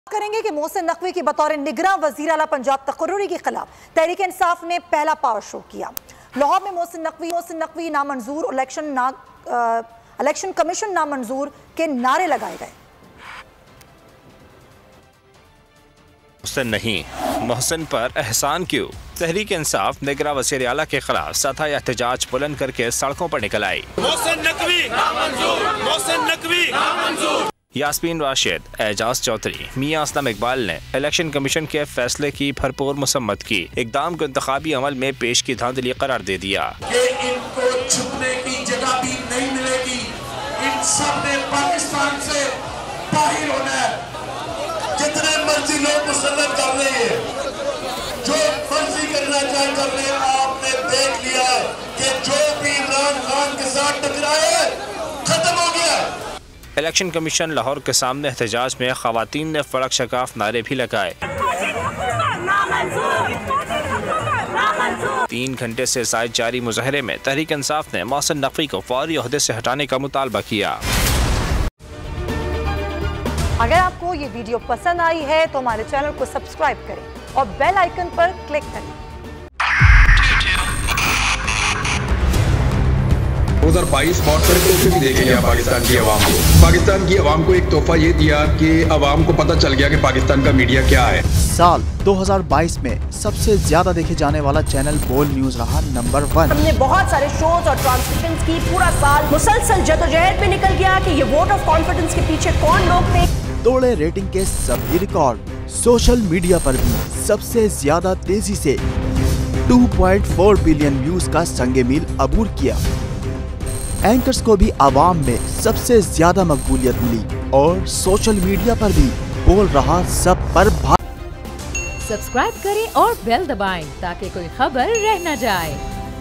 Mohsin Naqvi Disapproved | Tehreek-e-Insaf Power Show Across the Country | Breaking News
Mohsin Naqvi Nominated Caretaker CM Punjab | Election Commission Decision | Caretaker Of CM Punjab?